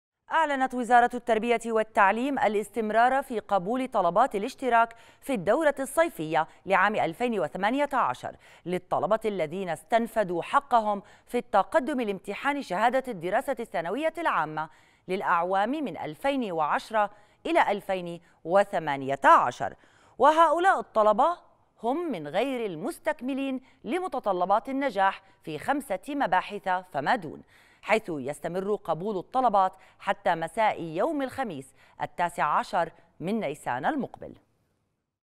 وزارة التربية توجه كتابا لإلغاء عقوبة الحرمان لطالبين في مدرسة خاصة - (22-3-2018)
وجهت وزارةُ  التربية والتعليم كتابًا الى إحدى المدارس الخاصة طالبت فيه بإلغاءِ عقوبةِ الحرمانِ من التعليم لطالبيْن, والاكتفاءِ بعقوبةِ الإنذار، وذلك على خلفيةِ اعتداءِ احدِ الطلبةِ جسديا على زميلهِ في أحدىِ المدارس الخاصة بعمان, وقيامِ طالبٍ آخر بتصويرِ الحادثةِ ونشرها كمقطع فيديو على مواقع التواصل الاجتماعي.
وفي سياقٍ اخر، اعلنت وزارةُ التربية والتعليم الاستمرارَ في قبولِ  طلباتِ الاشتراك في الدورة الصيفيةِ لعام الفين وثمانيةَ عشر للطلبةِ الذين استنفدوا حقَهُم في التقدمِ لامتحان شهادة الدراسة الثانوية العامة للأعوامِ من الفين وعشرة الى الفين وثمانيةَ عشر.
وهؤلاءِ الطلبةُ هم من غيرِ المستكملين لمتطلبات النجاح في خمسةِ مباحثَ فما دون، حيثُ يستمرُ قبولُ الطلبات حتى مساءِ يوم الخميس التاسعَ عشر من نيسان المقبل